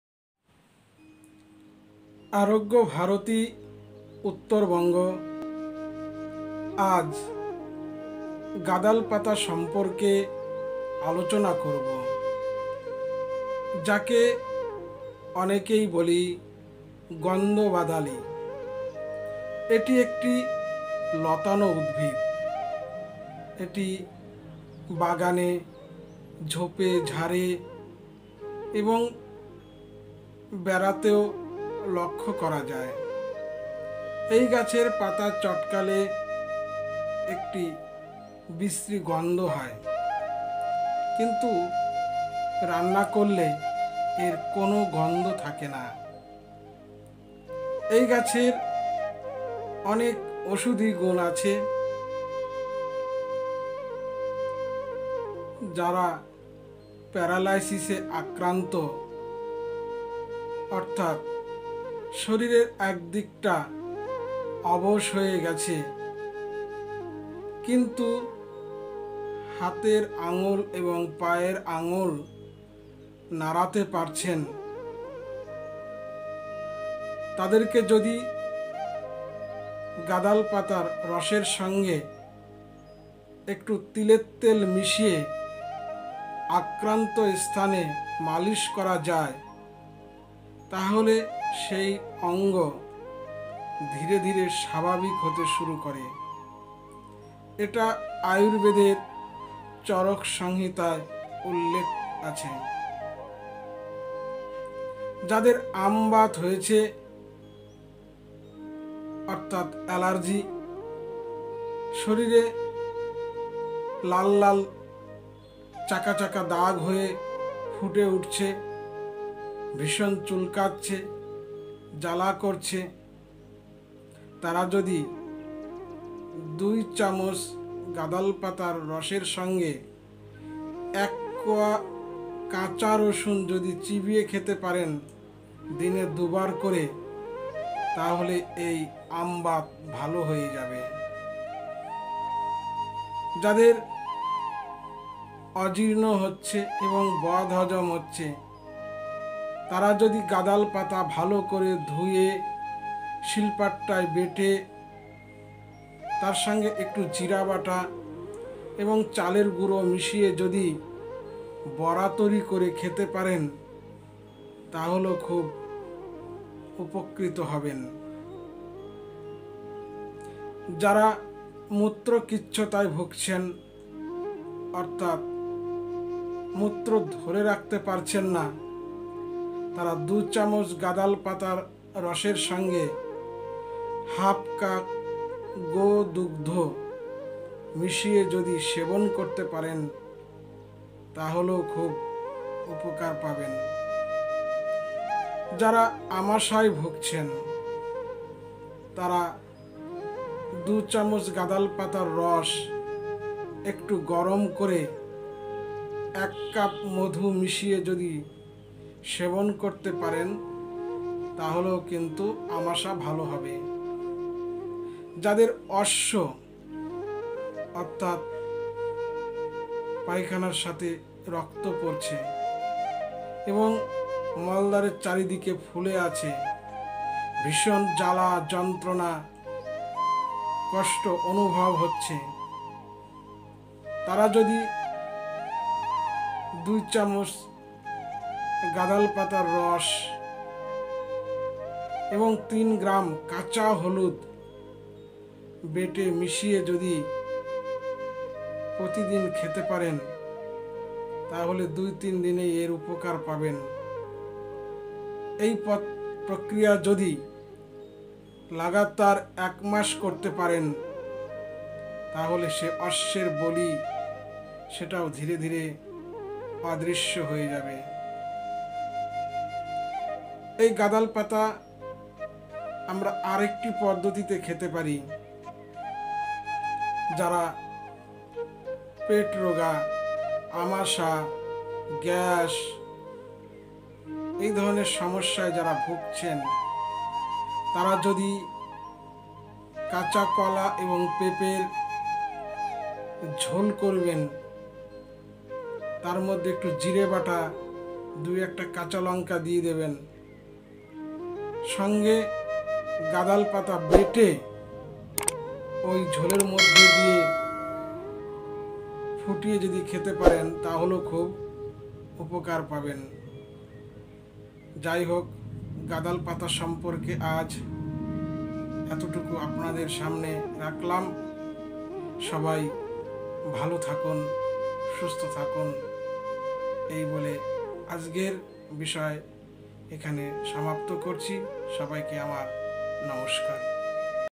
গাঁদাল পাতার উপকারিতা
গাঁদাল পাতা গ্যাস  বদহজম আমাশয় আমবাত অর্শ প্যারালাইসিস ভালো হয়